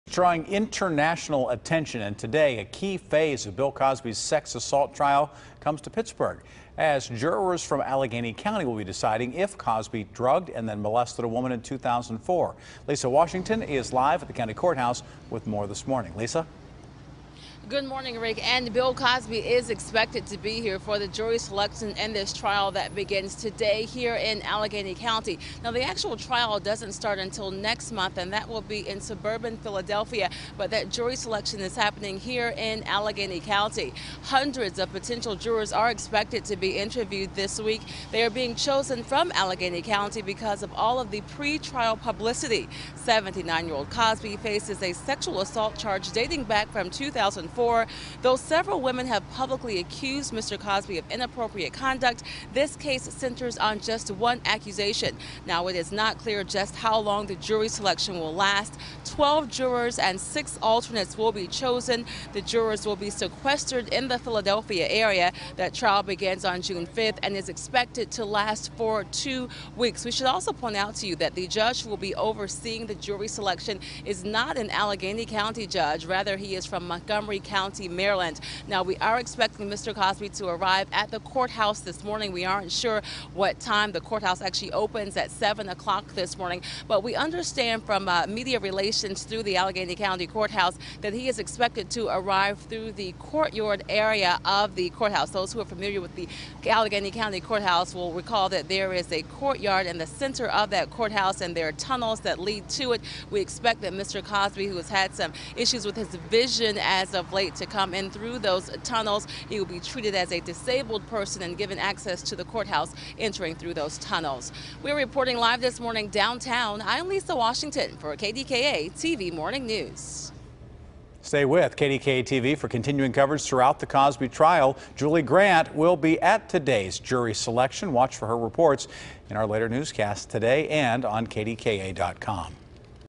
Jury Selection For Cosby Trial Begins Monday In Pittsburgh
A key phase of comedian Bill Cosby's sex assault trial starts Monday when jury selection gets underway in Pittsburgh.